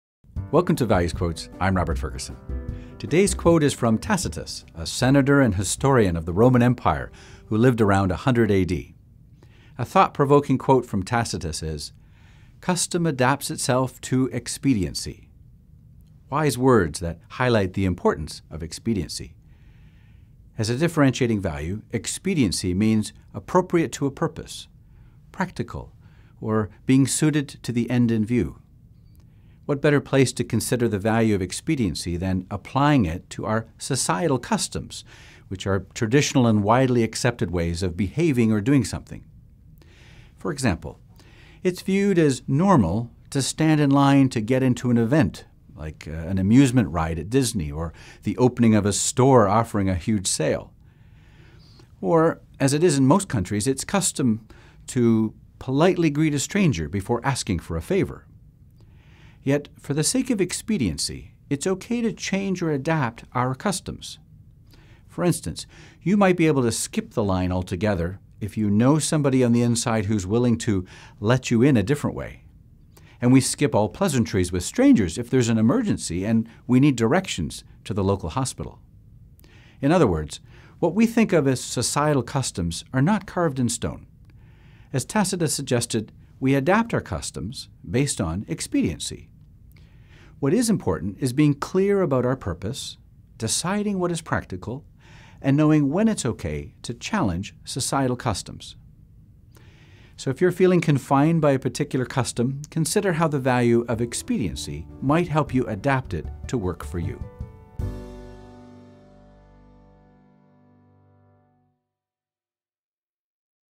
Tacitus on Expediency
- Todays quote is from Tacitus, a senator and historian of the Roman Empire, who lived around 100 A.D.

A thought-provoking quote from Tacitus is:
"Custom adapts itself to expediency."

Wise words that highlight the importance of expediency.

As a differentiating value, Expediency means appropriate to a purpose; practical; or being suited to the end in view.

What better place to consider the value of expediency than applying it to our societal customs, which are traditional and widely accepted ways of behaving or doing something. For example, it's viewed as normal to stand in line to get into an event, like an amusement ride at Disney or the opening of a store offering a huge sale. Or, as it is in most countries, it's custom to politely greet a stranger before asking for a favor.

Yet, for the sake of expediency, it's ok to change or adapt our customs.

For instance, you might be able to skip the line altogether if you know somebody on the "inside" who is willing to let you in a different way. And we skip all pleasantries with strangers if there's an emergency and we need directions to the local hospital.

In other words, what we think of as society customs are not carved in stone. As Tacitus suggested, we adapt our customs based on expediency. What is important is being clear about our purpose, deciding what is practical, and knowing when it's ok to challenge societal customs.

So, if you're feeling confined by a particular custom, consider how the value of expediency might help you adapt it to work for you.